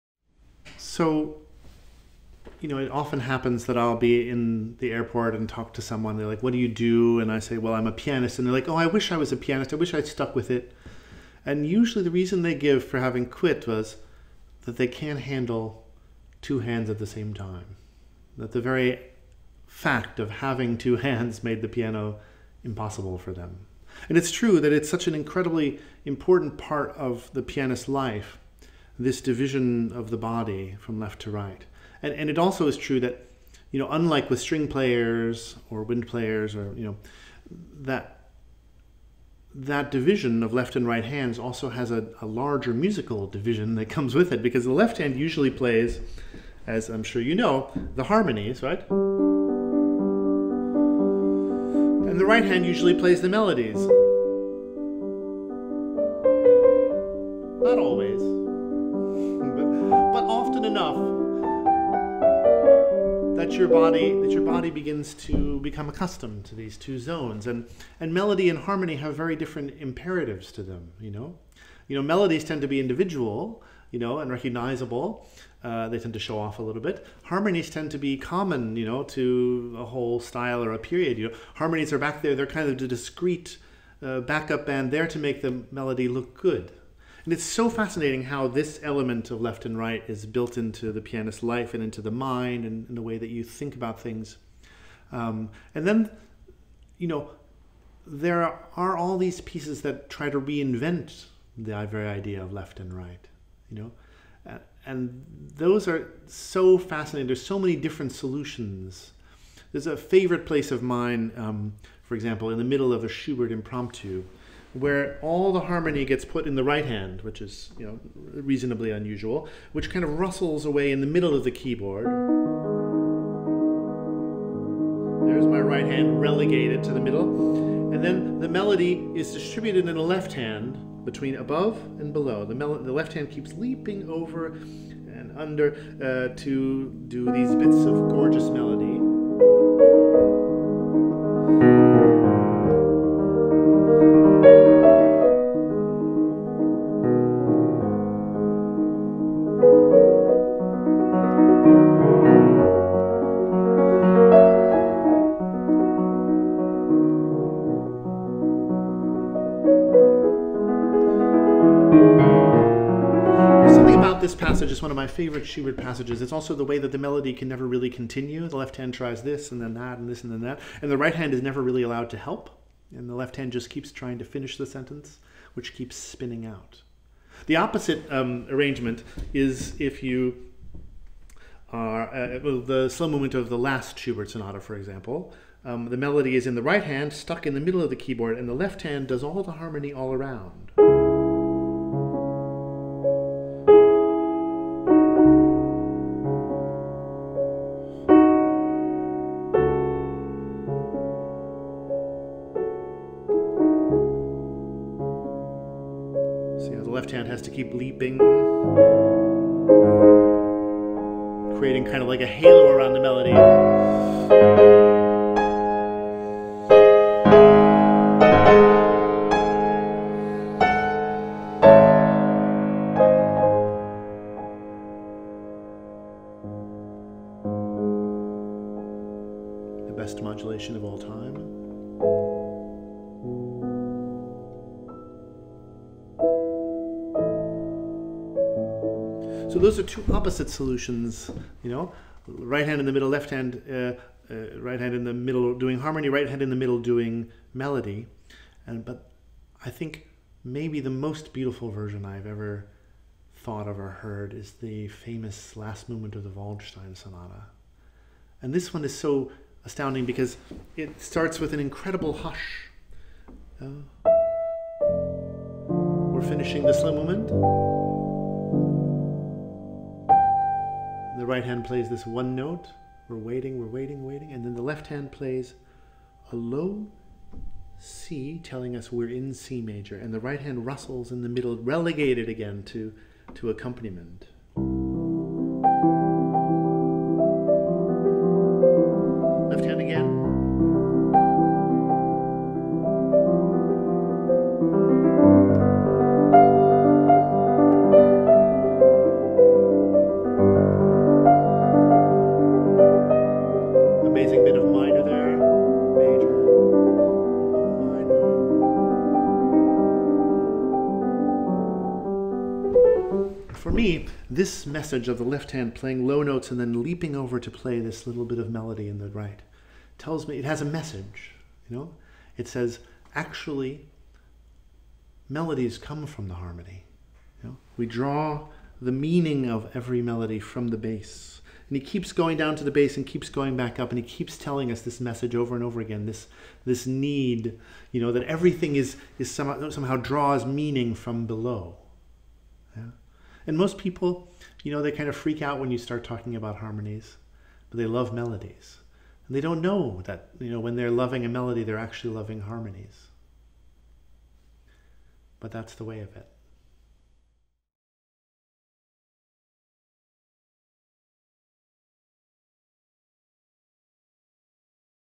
Jeremy Denk on Playing with Both Hands
Jeremy Denk, Macarthur “Genius” Award winning pianist and author of Every Good Boy Does Fine discusses playing piano with both hands.